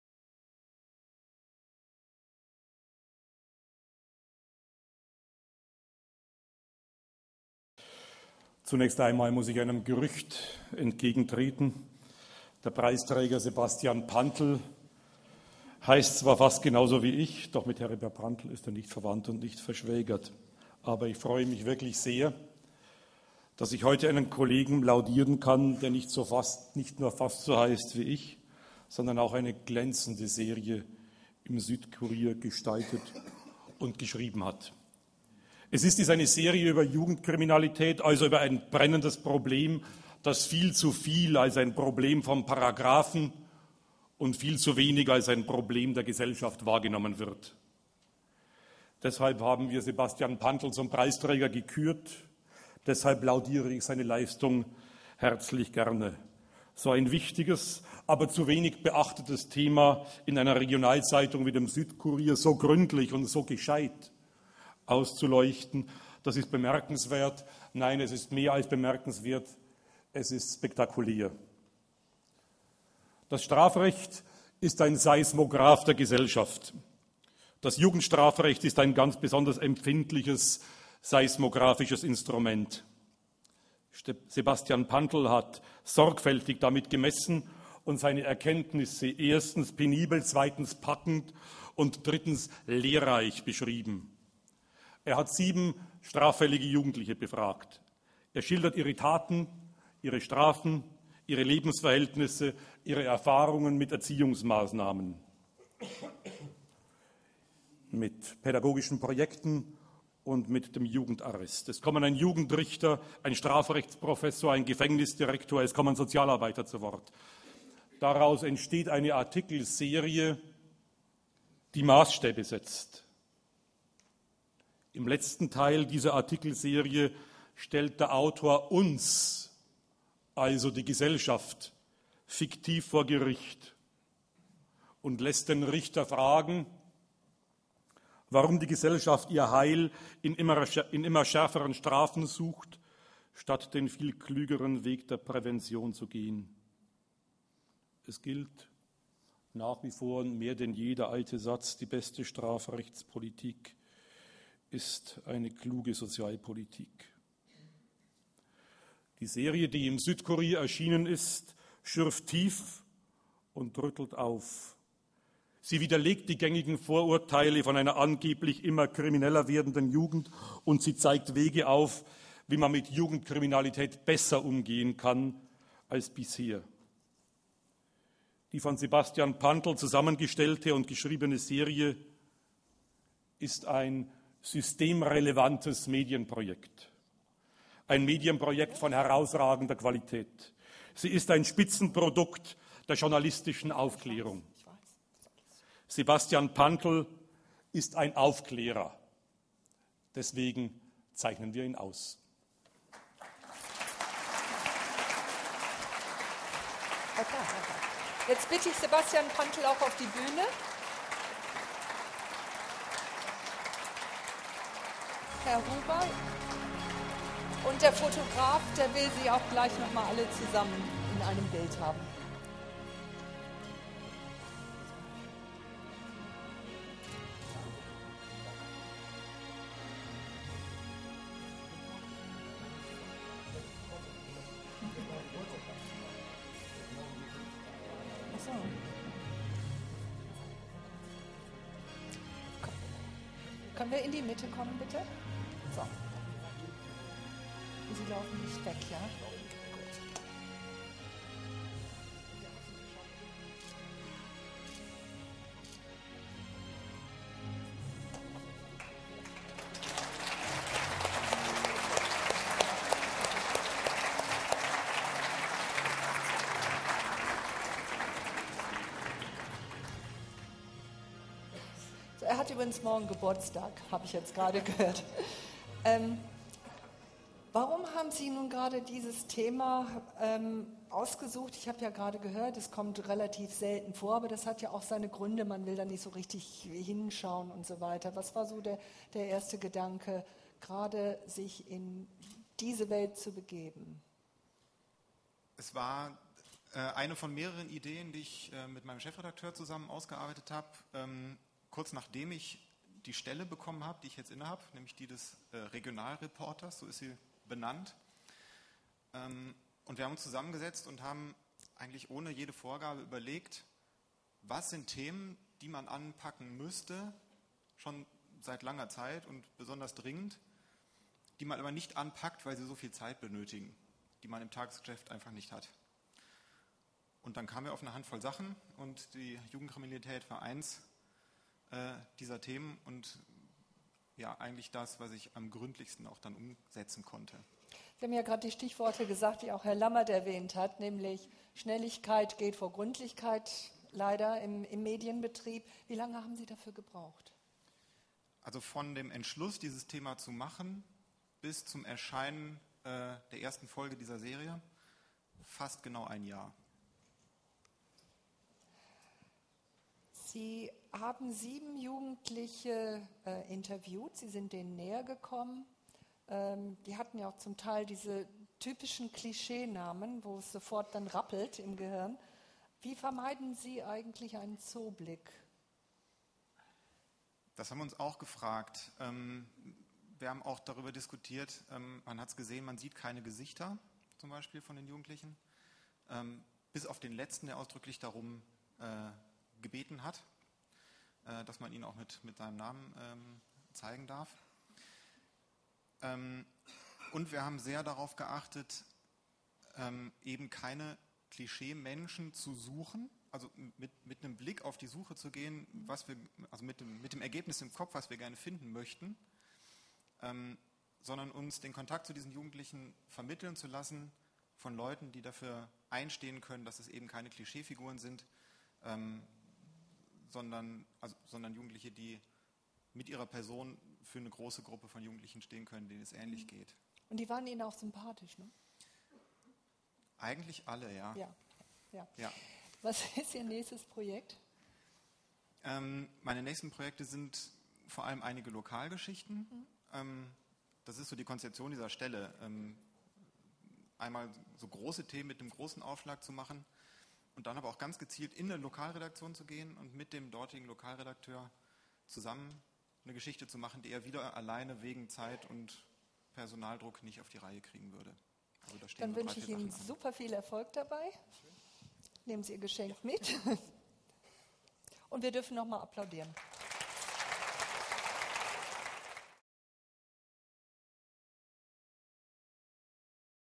Otto Brenner Preis - Medienprojektpreis - Laudatio von Heribert Prantl
Laudatio von Heribert Prantl und Preisübergabe an Sebastian Pantel, SÜDKURIER, für seine Artikelserie über Jugendkriminalität